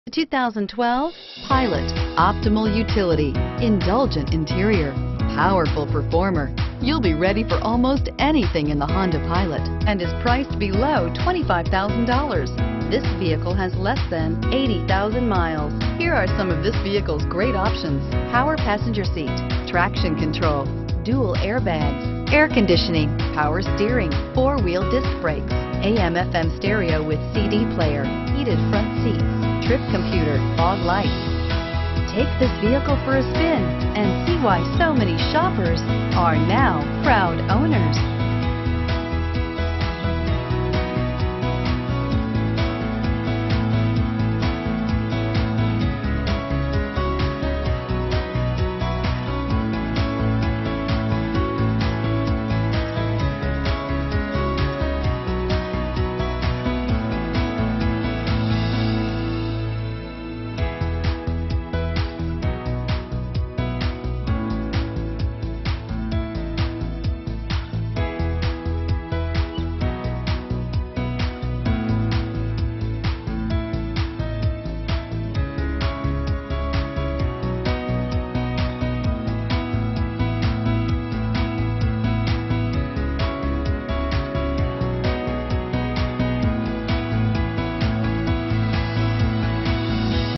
2012 Honda Pilot St. Charles IL J6115A
2012 Honda Pilot EX-L

1611 E. Main St.

4WD. The gas savings gives hybrids a run for their money. Move over gas-guzzlers, there's a new kid in town. When was the last time you smiled as you turned the ignition key? Feel it again with this charming-looking 2012 Honda Pilot.